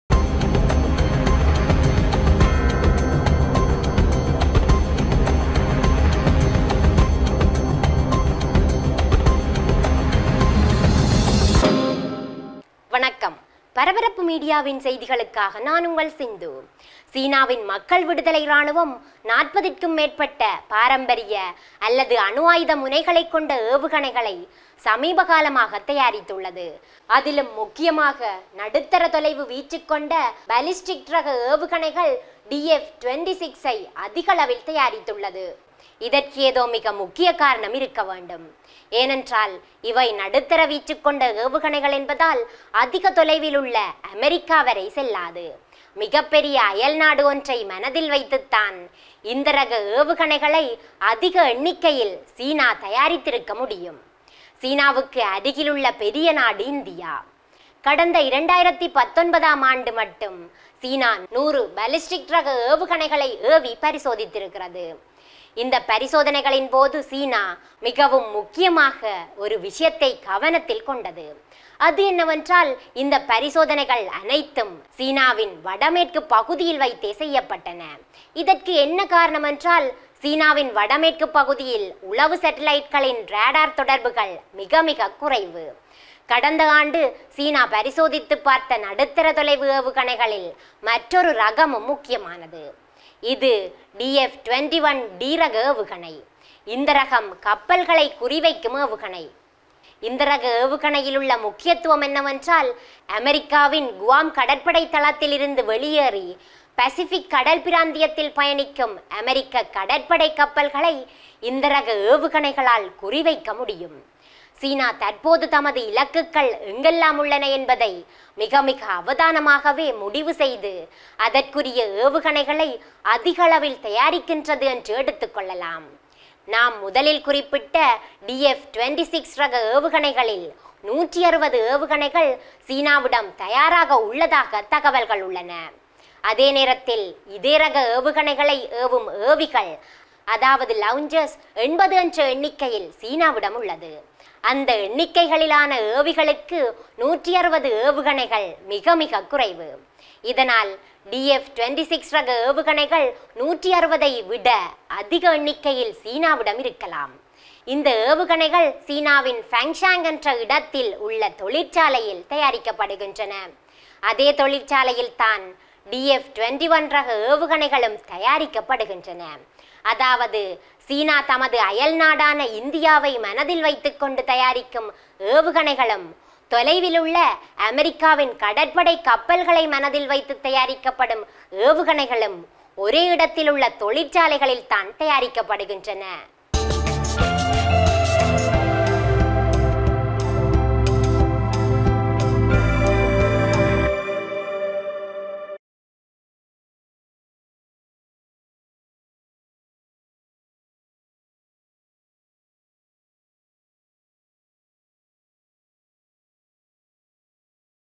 சீனா இந்தியாவையும் அமெரிக்காவையும் ‘மனதில் வைத்து’ தயாரிக்கும் ஏவுகணைகள்! | Paraparapu World News
One key type is the DF-26 intermediate-range missile, which is being introduced in greater numbers.China is not decelerating in any way its development of missiles.